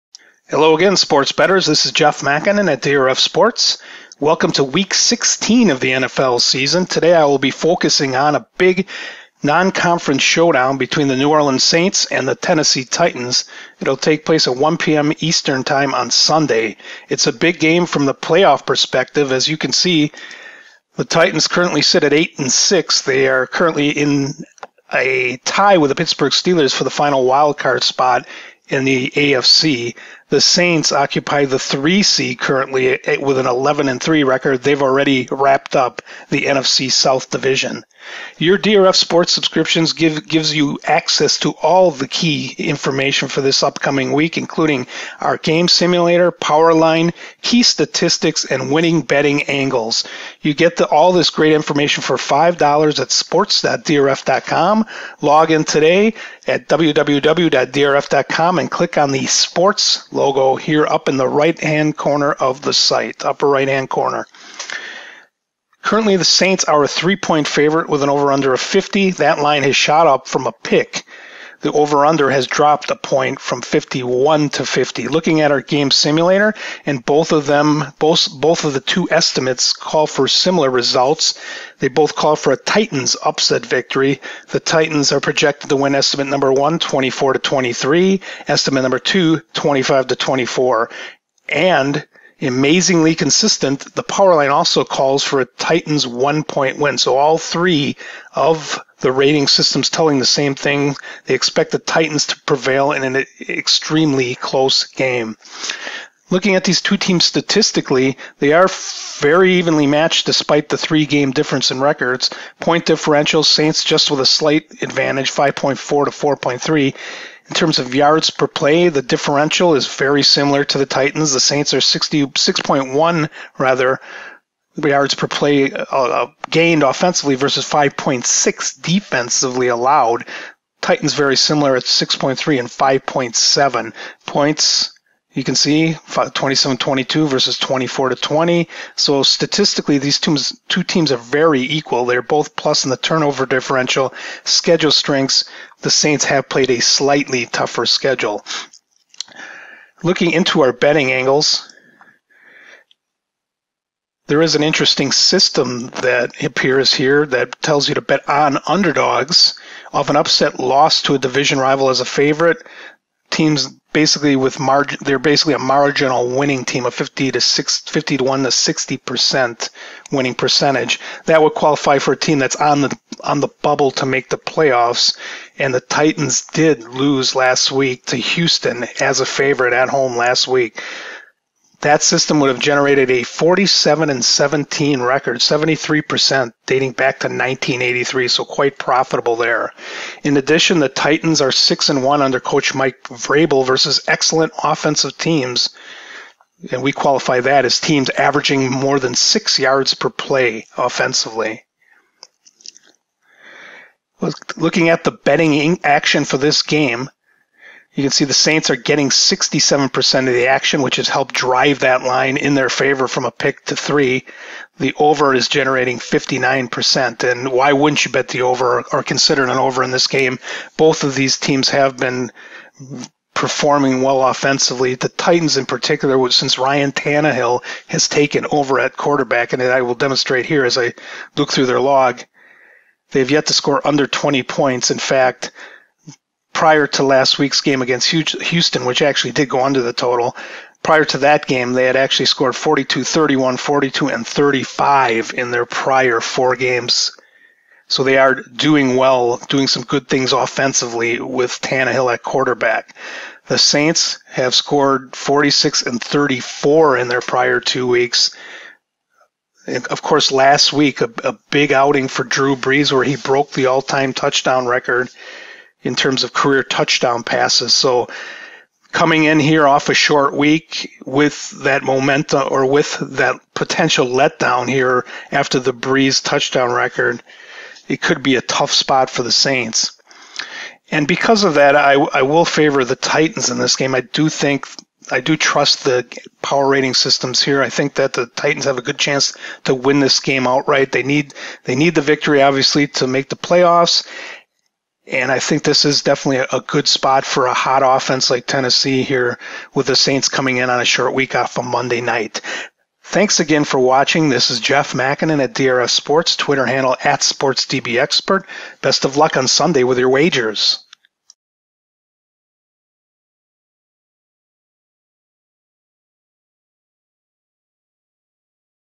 DRF Sports Game Preview - Saints vs. Titans - December 22, 2019
Here's Jeff Makinen with the preview of Sunday's match up between the Saints and Titans  using DRF Sports (powered by Statfox) on DRFTV.